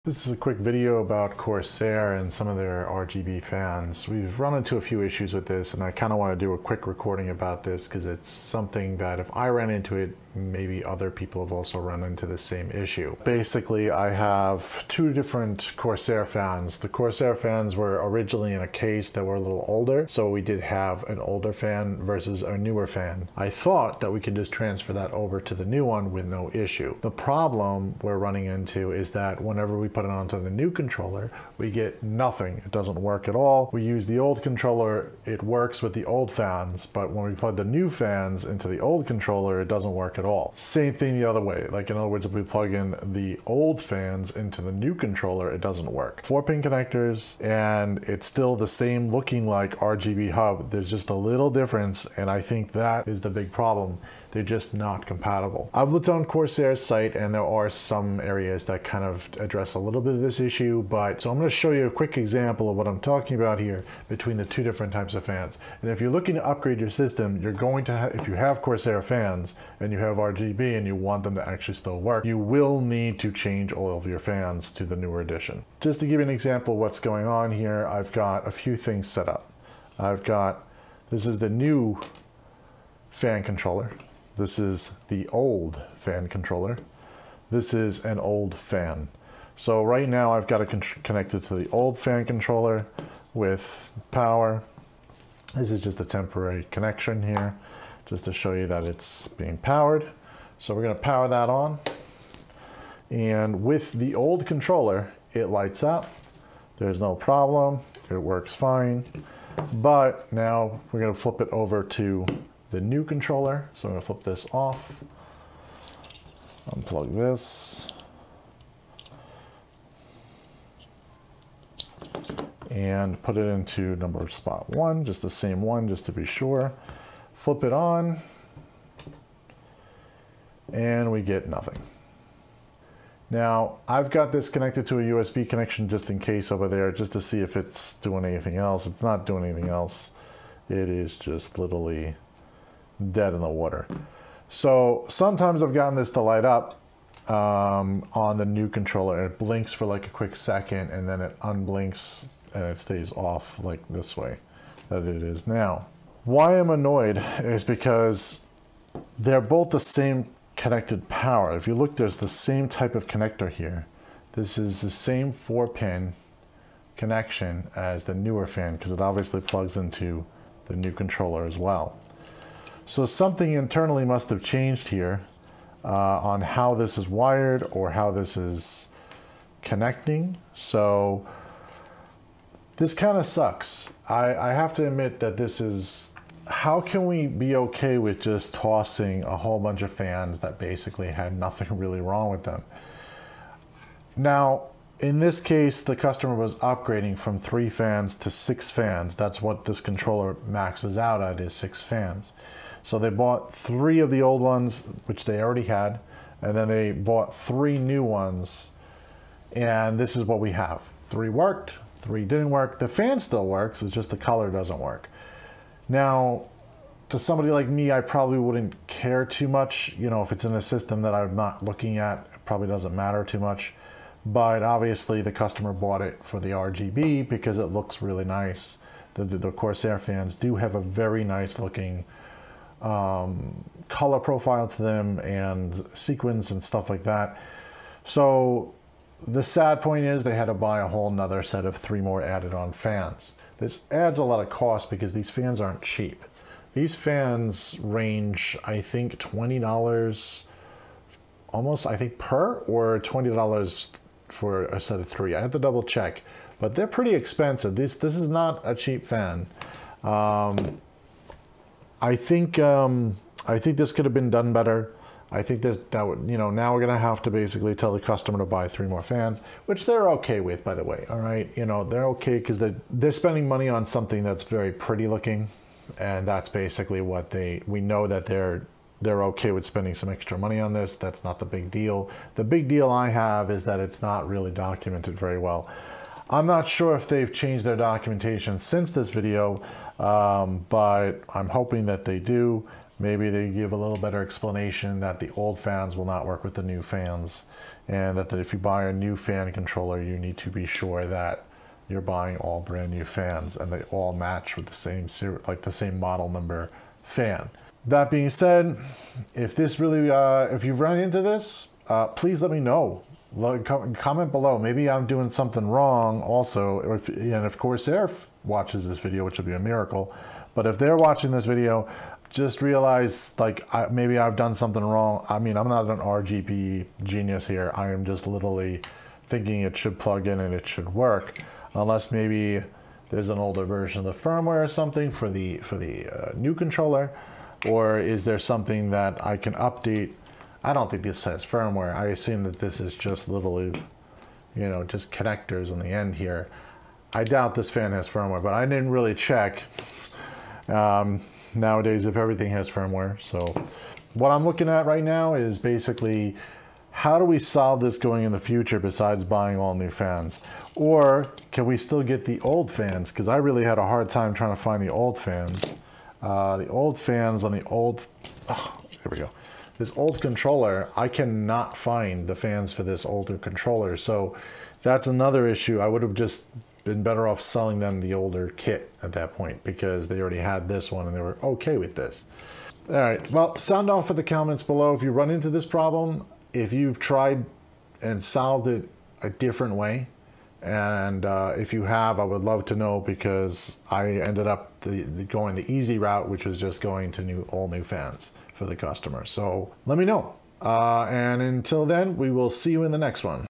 RGB Corsair Fan Problems - Old vs. New
We ran into a problem with Corsair RGB fans the older fans not working with the new controller.  If you have experienced this have a watch of this video and share your experience in the comments below.